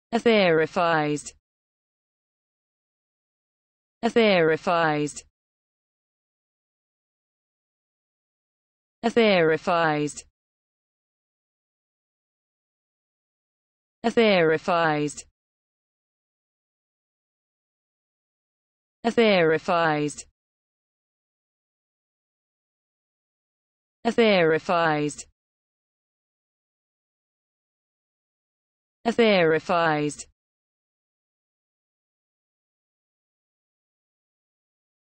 the correct pronunciation of etherifies in English.
This video shows you how to pronunciation etherifies in English.